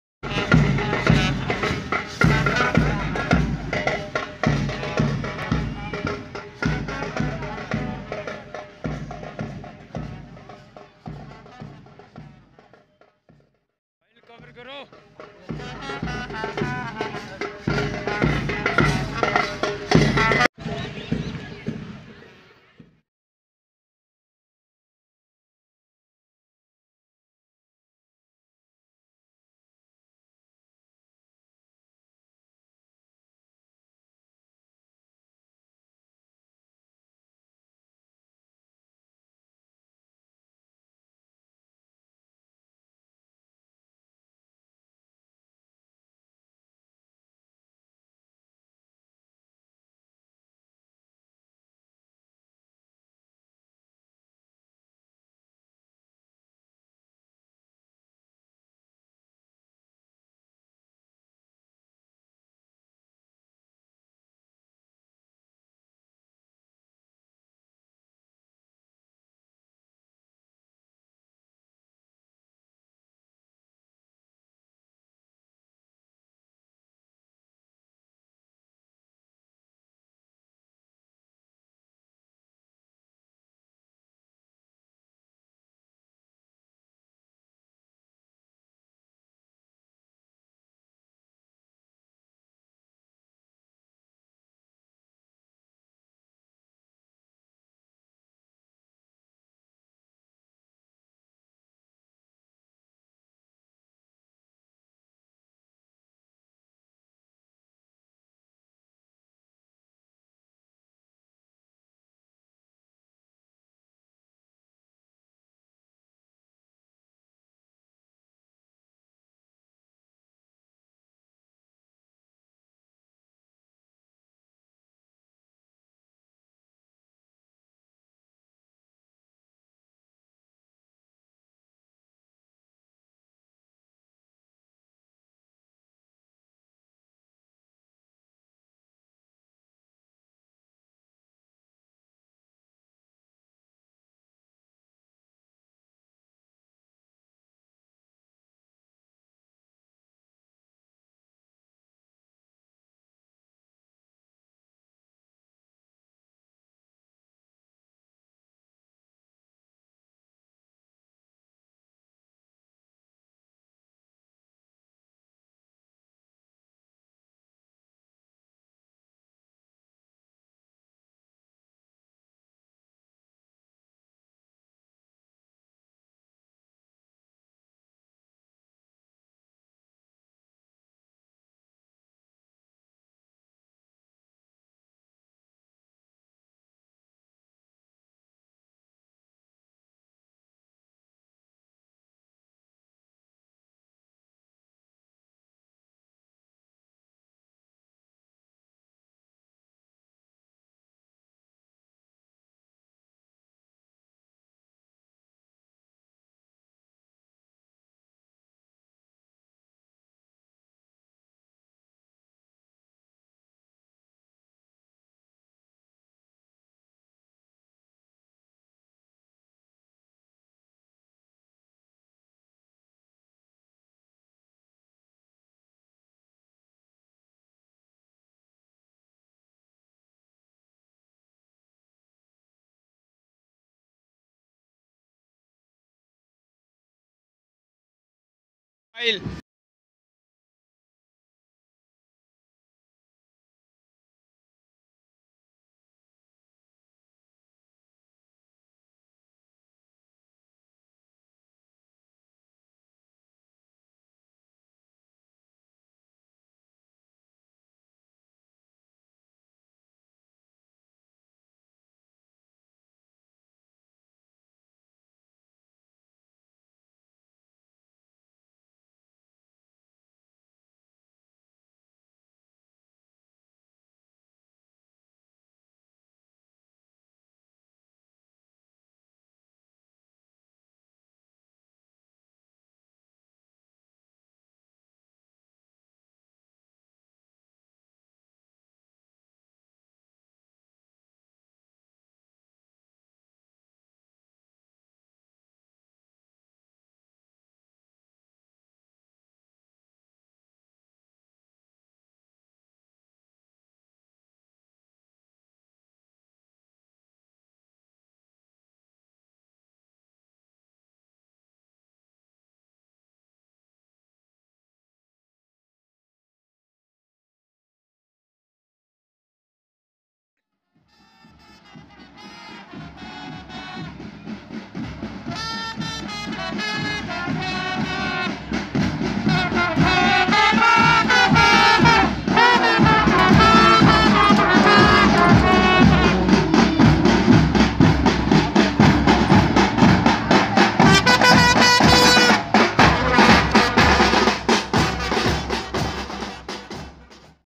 nawalgarh :- सूर्य मण्डल में 15 अगस्त की परैड की तैयारी हुई शुरू
वहा पर - पोदार कॉलेज एनसीसी, पोदार स्कूल एनसीसी, डिपीएस स्कूल, एस. एन. सीनियर सैकेण्डरी स्कूल. आदी मौजूद थे